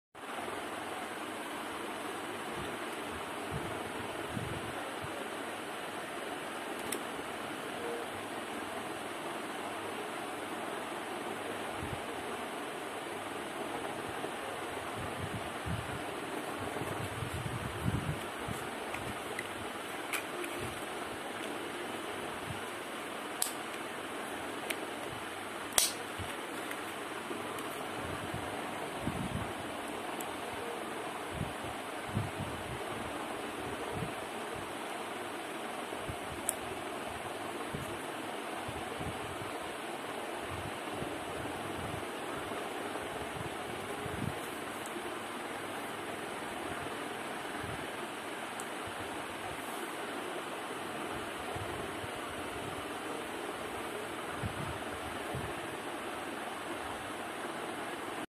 Best Otg Pendrive? SanDisk Ultra Dual USB 3.0 New pack at 740 price Wroth or waste money
Amazing Otg mobile pendrive with 150MB/s speed.